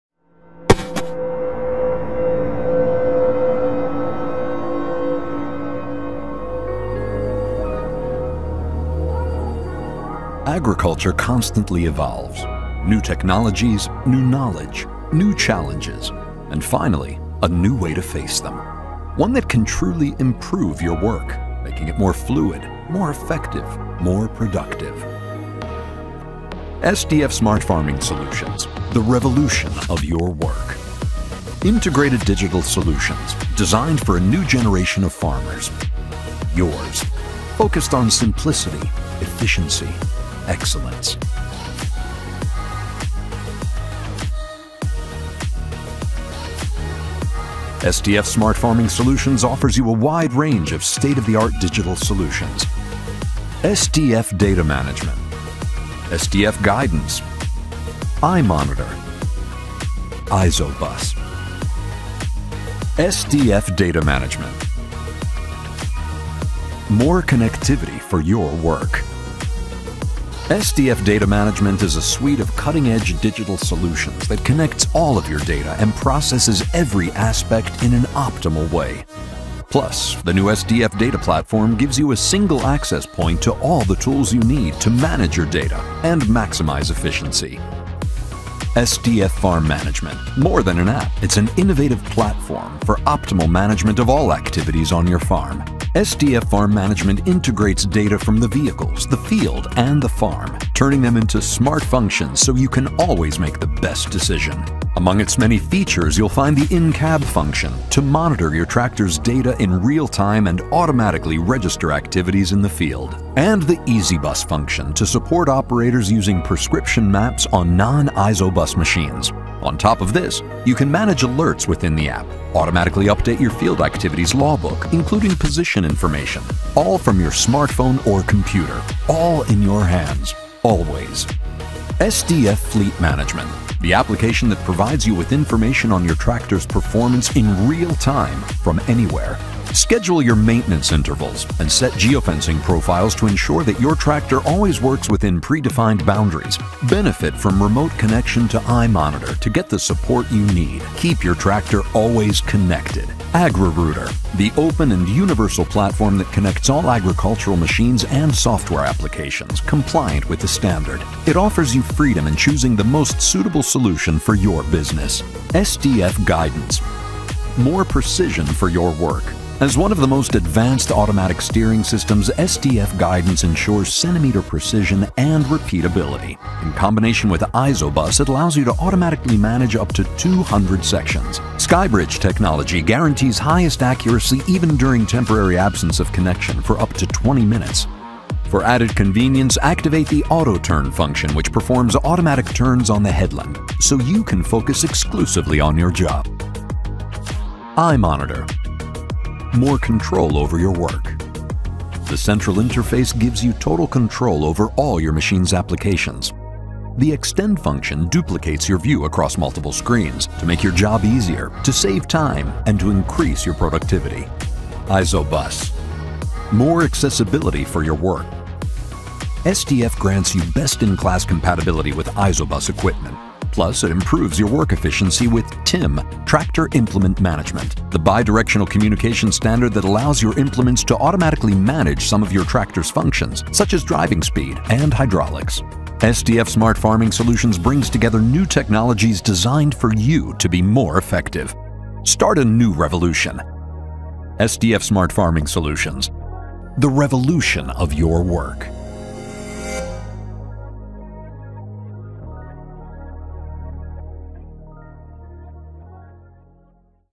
SDF Smart Farming Solutions. The revolution of your work.
SDF Smart Farming Solutions help you work even better than ever with a suite of tailorable digital solutions helping you make the best choices possible for your business, work more quickly and with more precision, and optimise your profitability.

SDF Smart Farming Solutions let you choose the package of services best suited to the needs of your business and keep all your decision-making and data under control.

We offer products and services perfectly suited to the practical needs of today’s farmers, contractors or other agricultural operators.